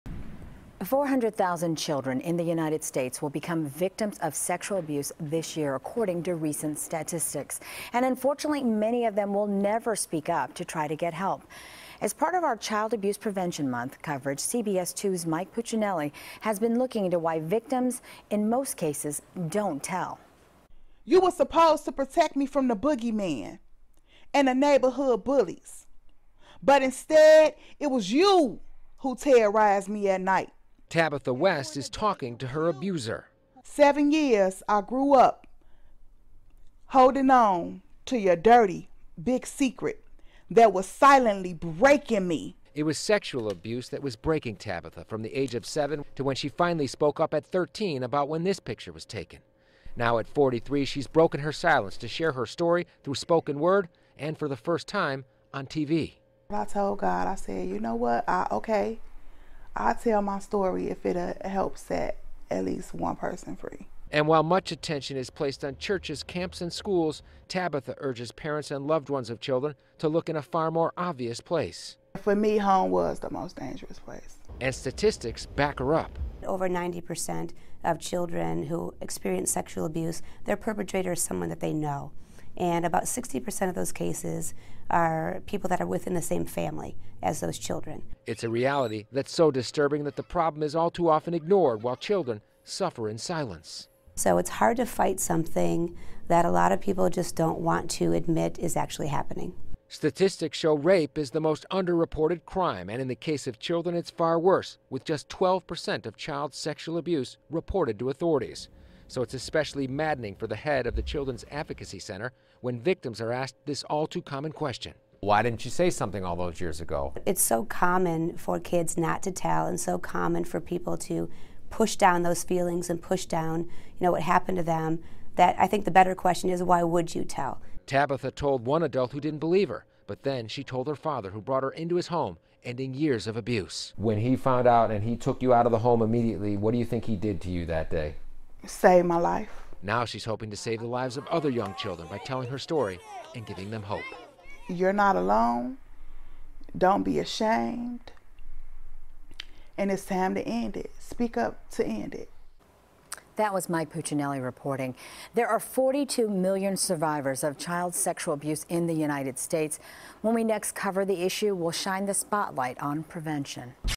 Sexual Assault Survivor Uses Spoken Word To Share Her Story
Tabatha Wells says she was abused from the age of 7 until she finally spoke up at age 13.